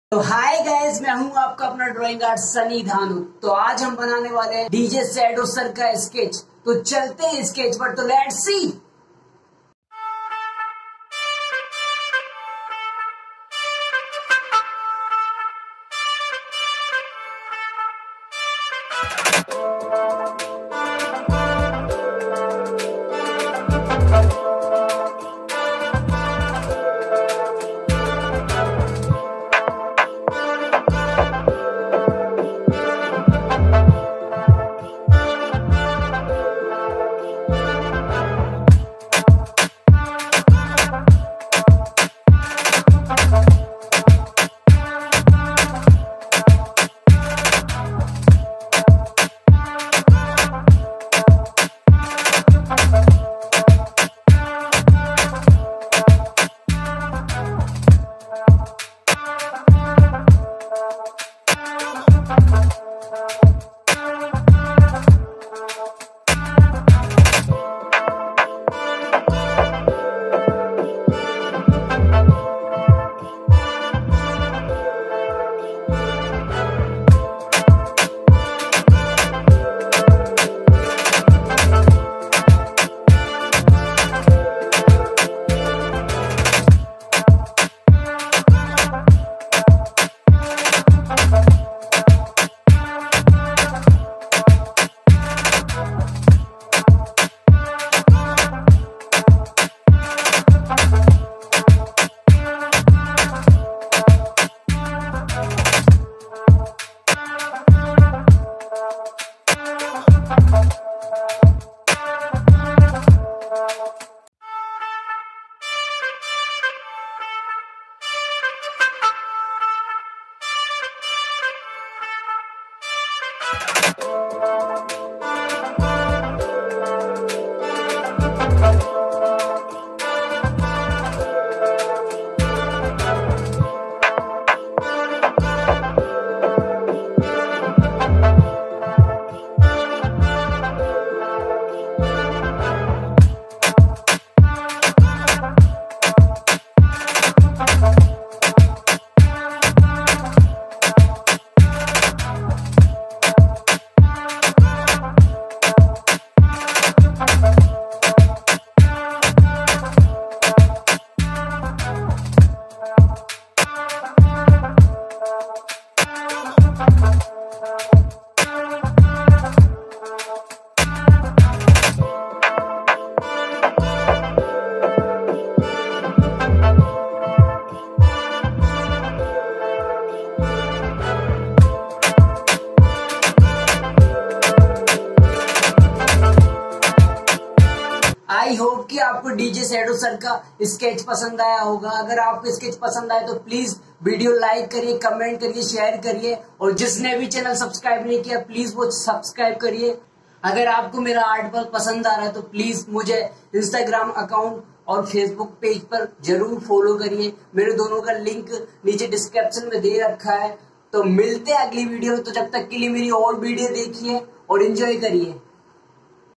Dj shadow dubai sketch video || how to draw dj shadow dubai || by shani dhanuk
My favorite Dj in the world... Only DJ shadow dubai sir 😍😍😍😍😍😍😍😍😍

1.grphite pencil

2.sharpner 3.charcoal pencil

5.My phone

About channel In your channel drawing art shani dhanuk you will get all kinds of information related to drawing and sketch and other arts.